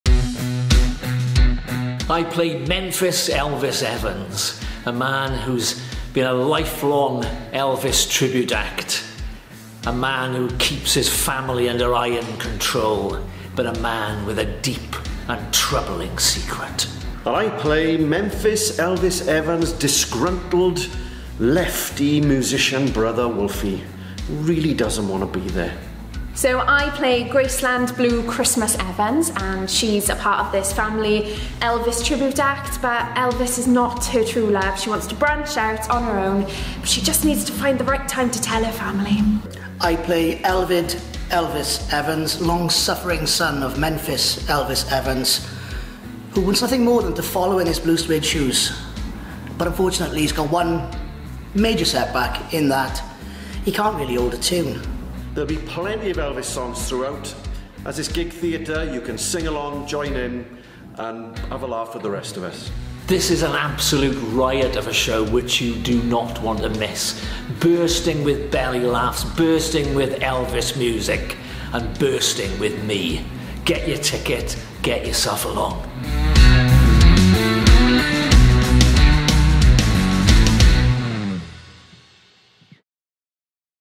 Elvis: The Rhondda Years at Porthcawl Elvis Festival 2024!!
Memphis Elvis Evans and his family have considered themselves the best Elvis Tribute Act in South Wales for years. It is the day before the Elvis Festival in Porthcawl - the biggest moment of every year for them. But all is not well, there’s rebellion in the ranks, as old secrets and crazy jealousies, as well as a nasty burn from a Greggs’ pasty at just the wrong moment, could tear them apart. This hilarious, rude, original piece of gig theatre - filled with live songs and bursting with energy is on its way to Porthcawl Elvis Festival 2024!

 Elvis, is once more, in the building!

Book now!!